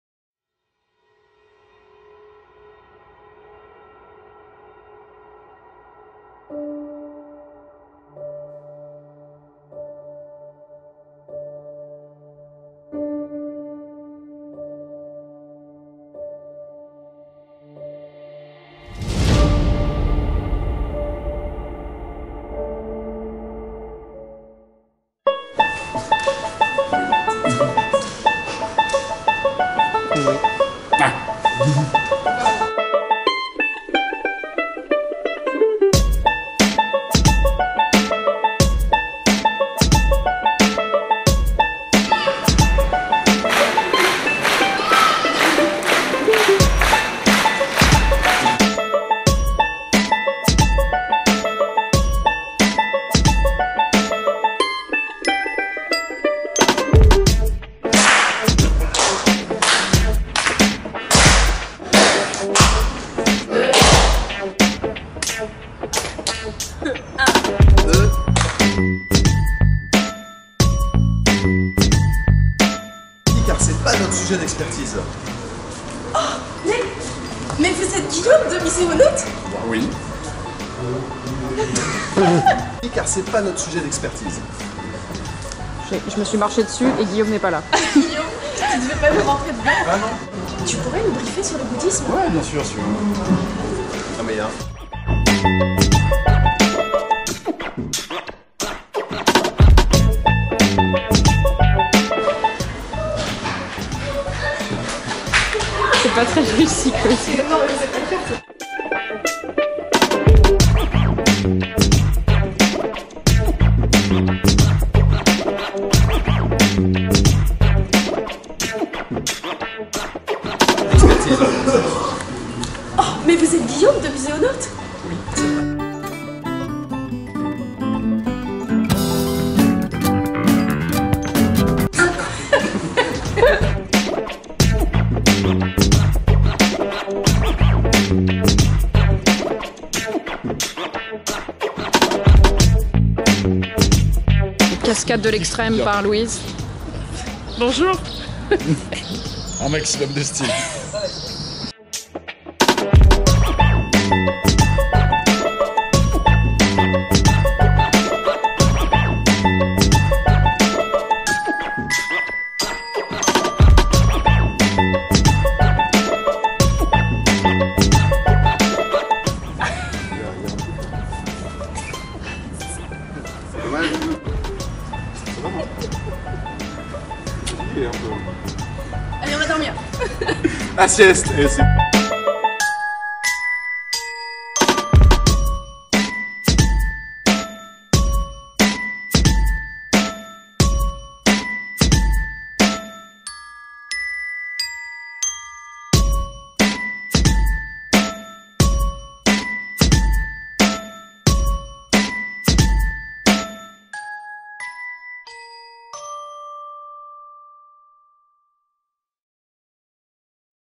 MAKING OF Osmosis & Muséonaute tournent Enfers & Fantômes d'Asie!
ET BIM! Grâce à Sweetberry, notre cadreuse de l'extrême, on a un petit souvenir de tournage à vous montrer!

C'était pas facile mais on s'est bien amusés quand même 💪